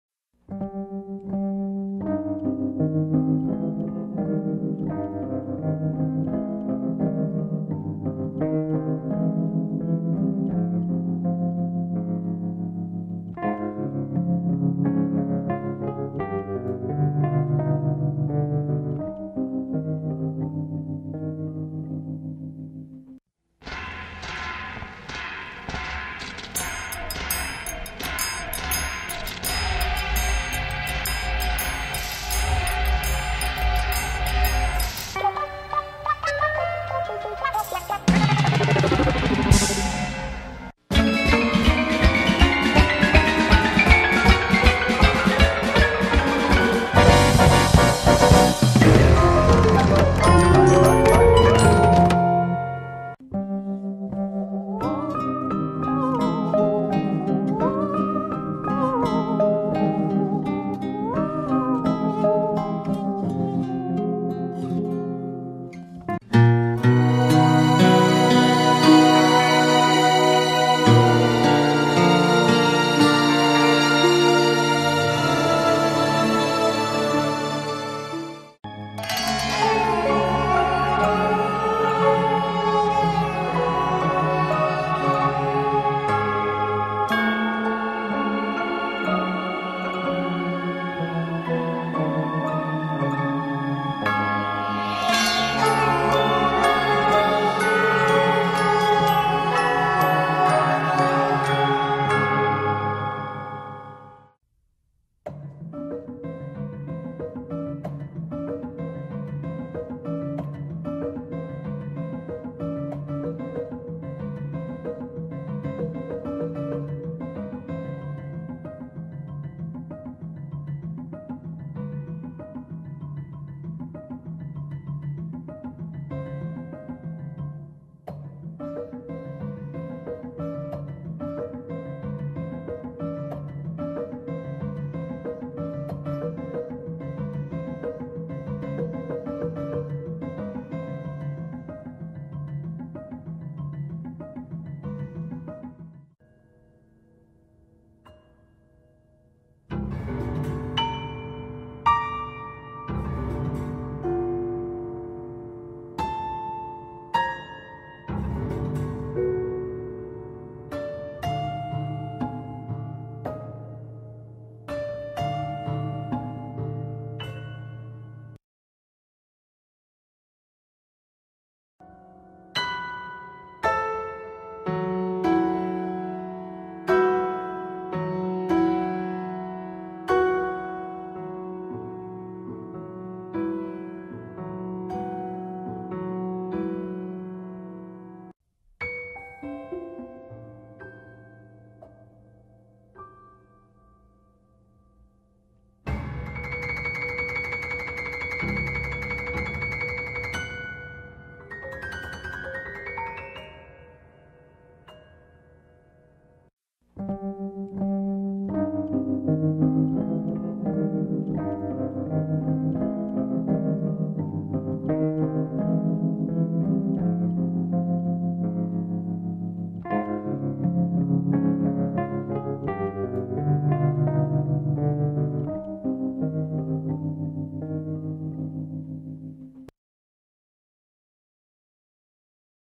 The Aeolian Harp
Stop motion animation of the myth of Orpheus by me I made it.